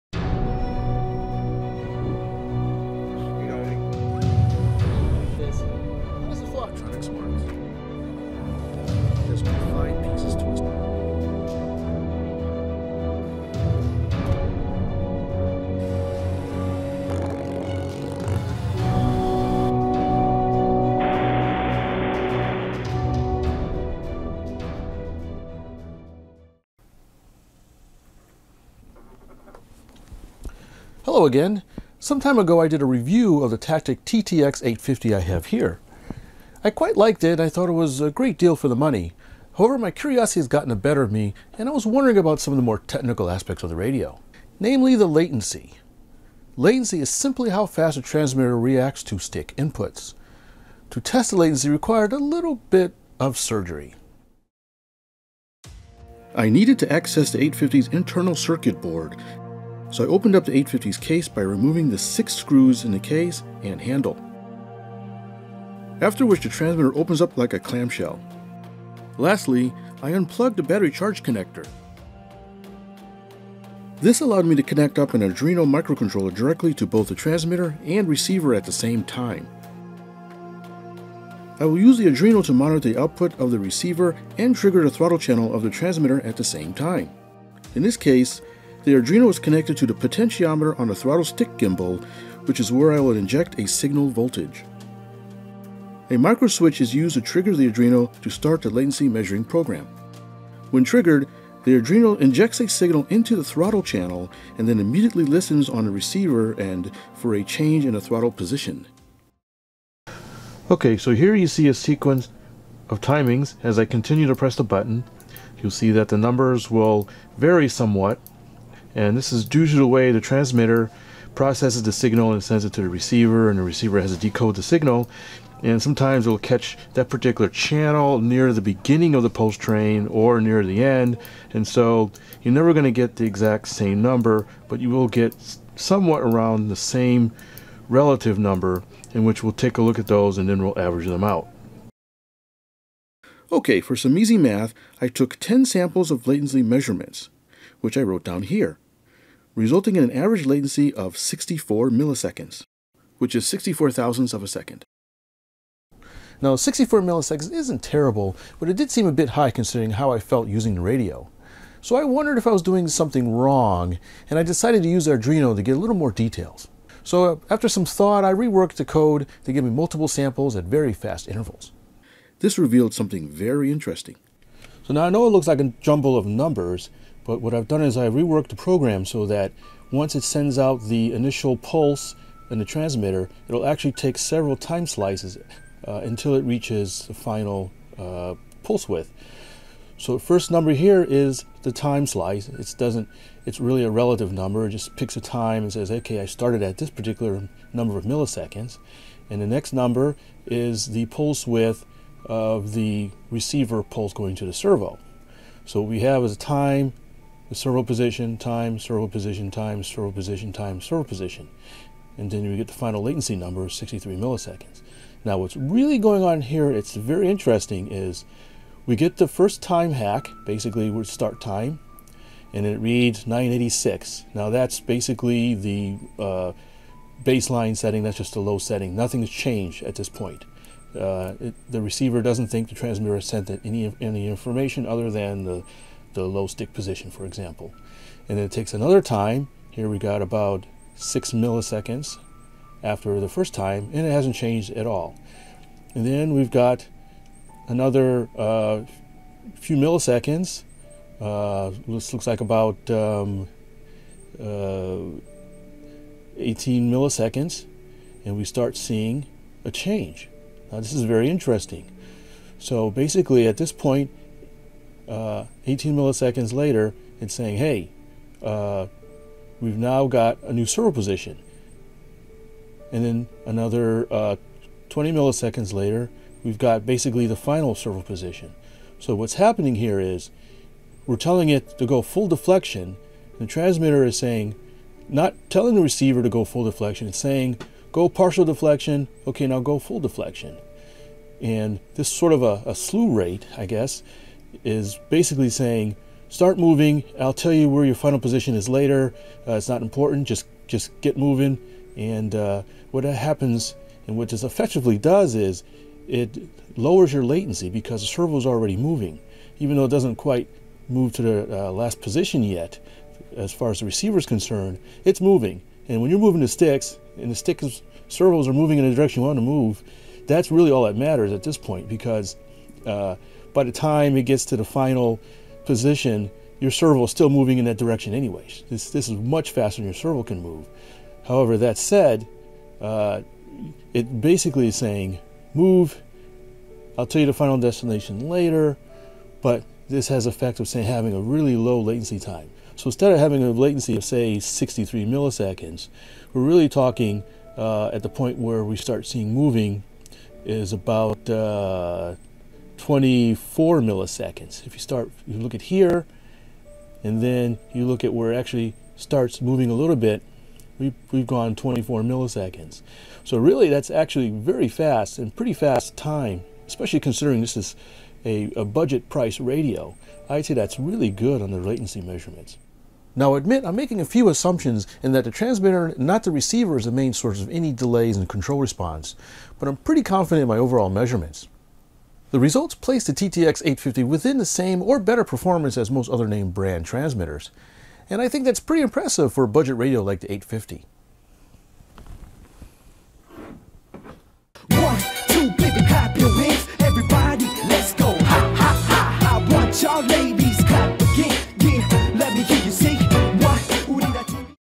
Tactic TTX850 Latency Tests | HobbyView
Something a little different this time.
They told me there would be NO MATH. Well there is a little bit of math in the episode, but most of it is done by computer, all you have to do is follow the bouncing ball as I hack a Tactic TTX850 to see how fast it is.